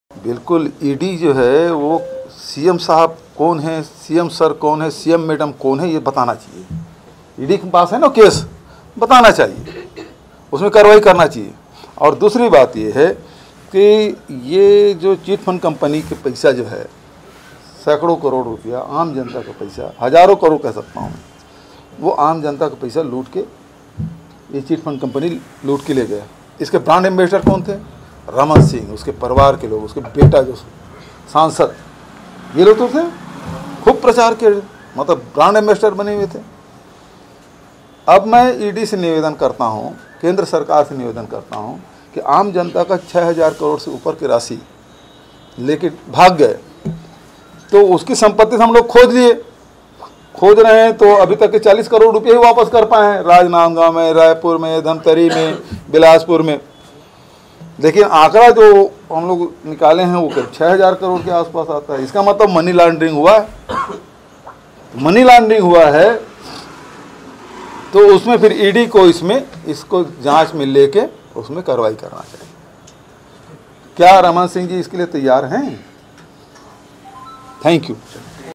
ई डी को बताना चाहिए कौन है CM सर और CM मैडम | Hamar Bhupesh Kakka
रमन सिंह और उनका परिवार
चिटफंड के ब्रैंड एम्बेस्डर